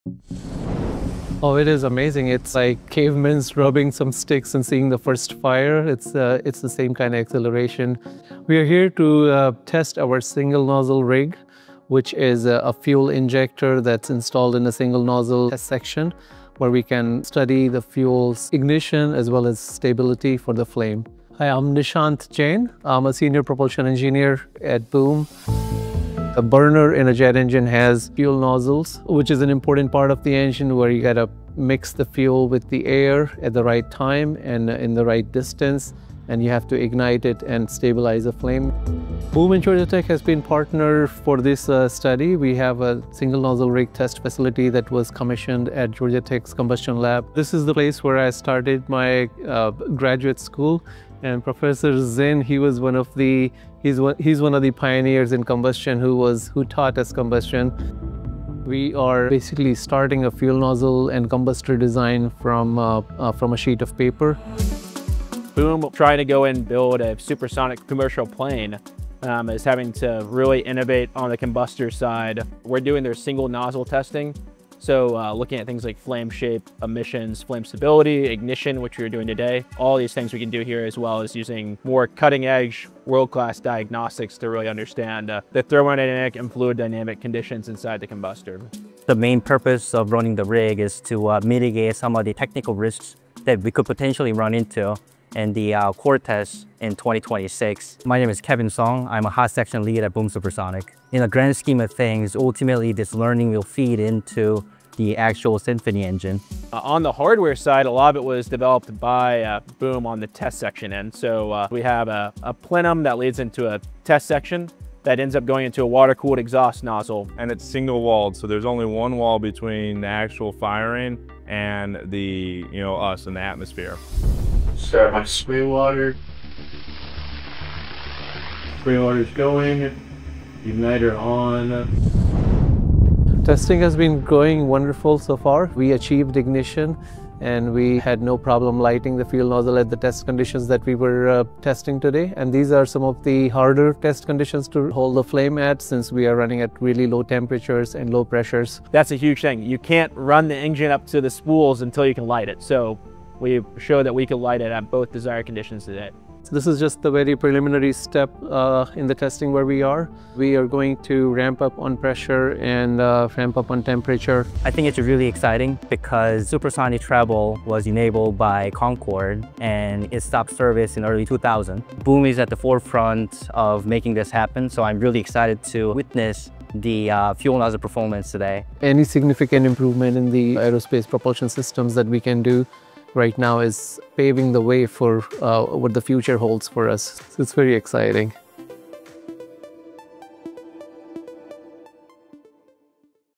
Inside the Flame: Testing Boom’s Supersonic Engine Ignitor
Go inside the Georgia Institute of Technology lab where engineers from Boom Supersonic test Symphony’s ignitor nozzle for the first time. This test marks a major milestone in the development of Symphony, the engine designed from the ground up for sustainable, supersonic flight.

00:00 Fuel Nozzle Rig Testing
00:26 Fuel Nozzles in Jet Engines
1:00 Building a Combustor From a Clean Sheet
1:13 Studying Flame Shape
1:41 How Testing Leads to Symphony Engine
1:47 Closer Look at the Test Rig
2:08 Watch the Combustion Test!
2:37 What's Next for Testing